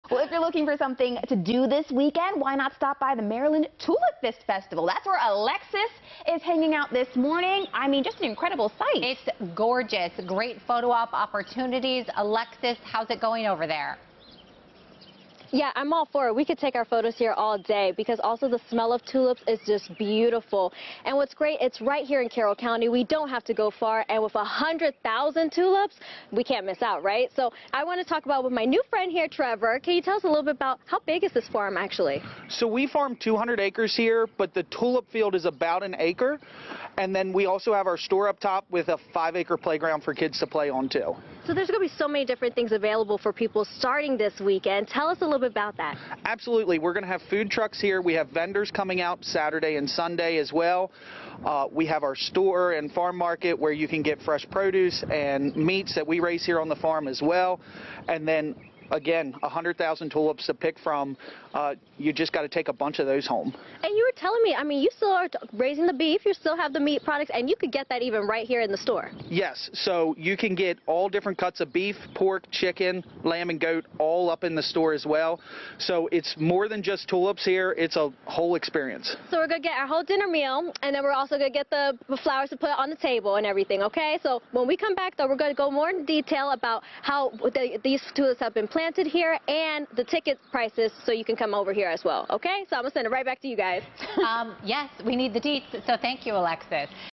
The Maryland Tulip Festival is this weekend in Carroll County
The first ever Maryland Tulip Festival continues this weekend, April 12th at Local Homestead Products in New Windsor, Maryland.